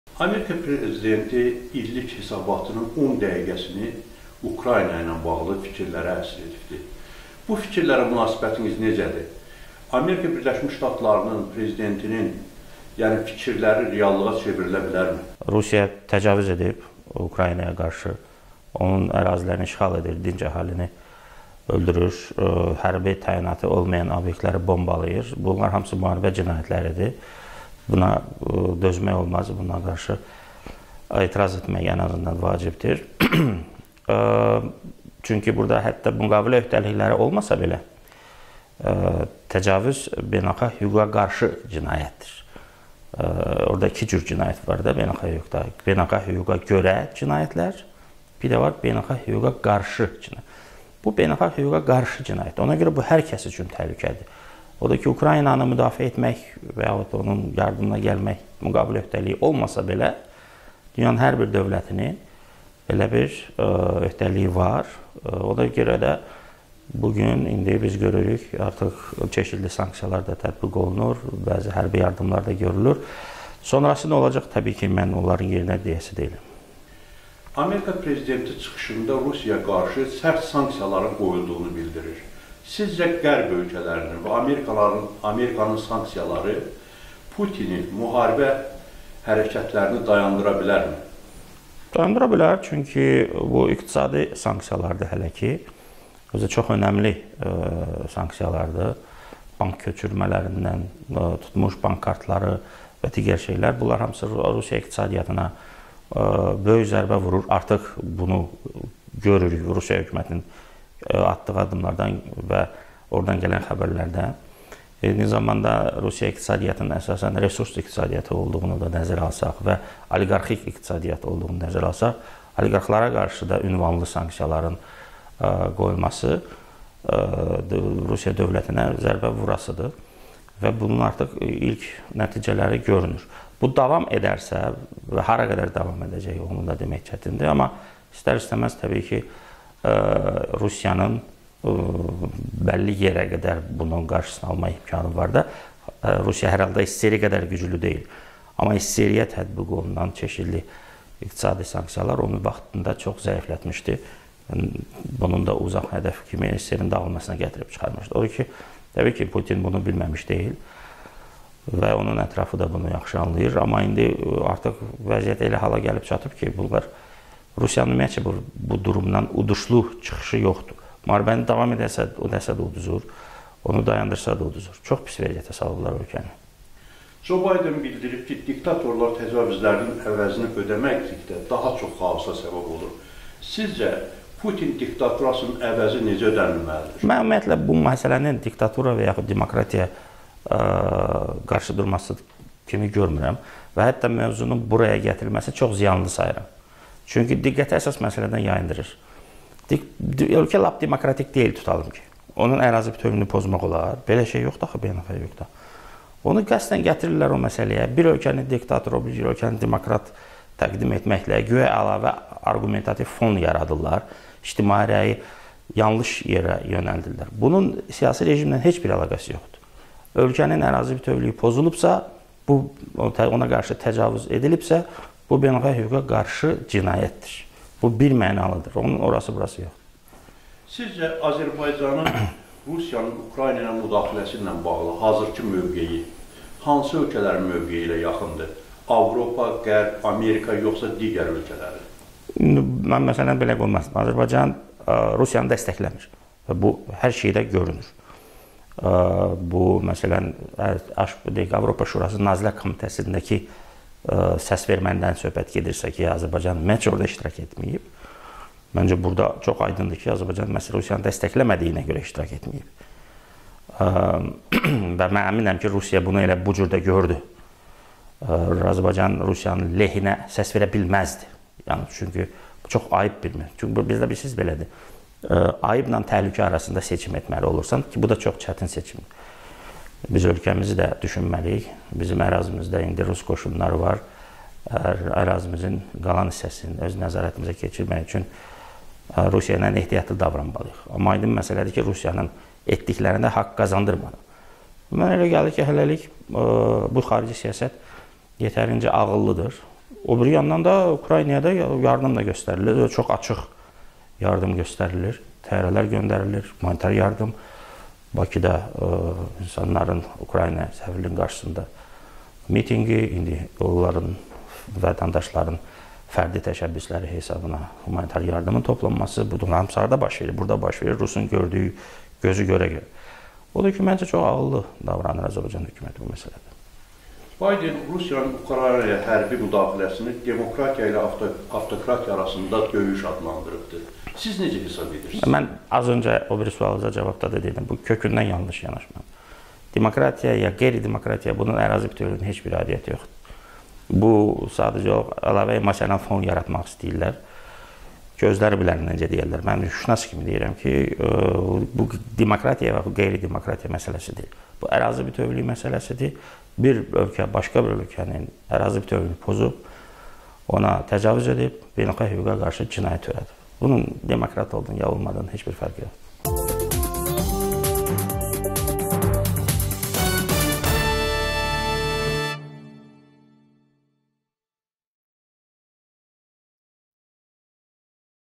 Erkin Qədirli: Rusiyanın uduşlu çıxışı yoxdur
Milli Məclisin deputatı Erkin Qədirli Amerikanın Səsinə müsahibəsində bildirib ki, Rusiya Ukraynaya qarşı təcavüz edib.